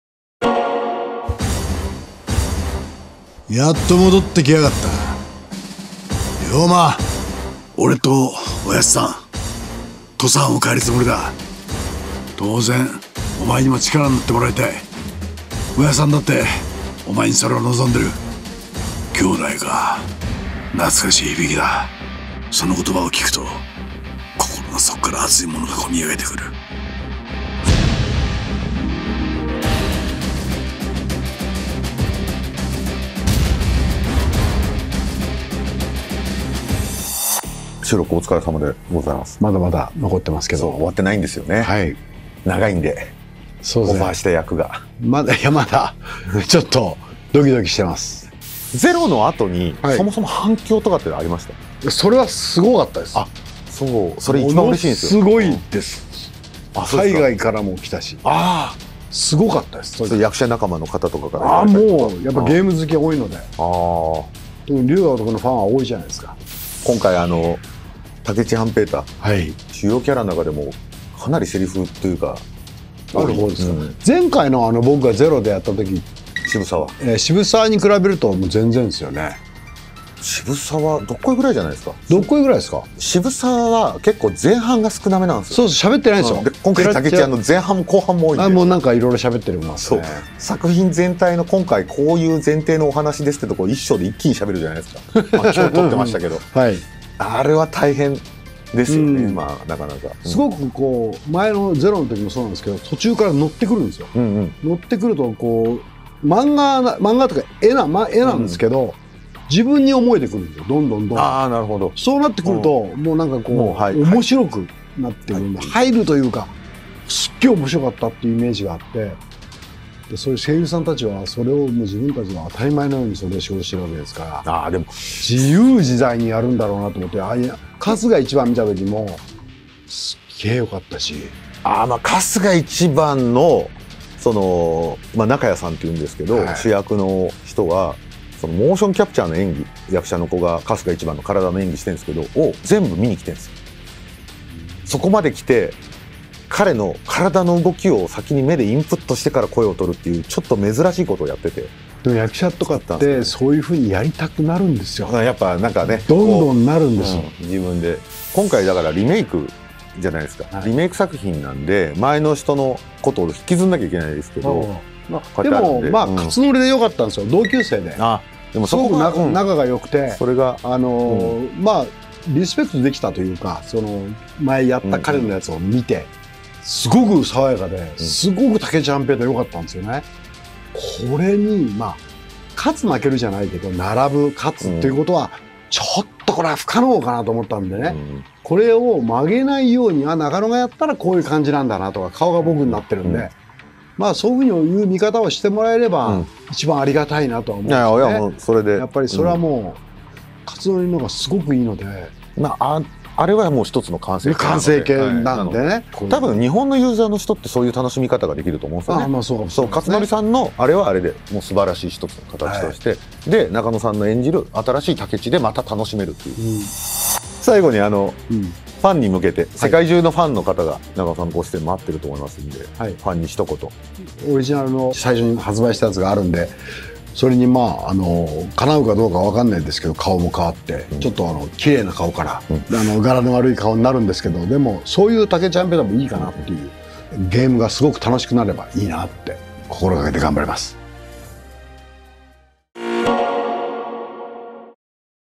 Like a Dragon: Ishin! Heated Discussions Nakano Hideo
Video interview with Hideo Nakano, who plays the role of Takechi Hanpeita, Ryoma's sworn brother and revolutionary leader!

Like a Dragon: Ishin!
2/22/2023 release confirmed!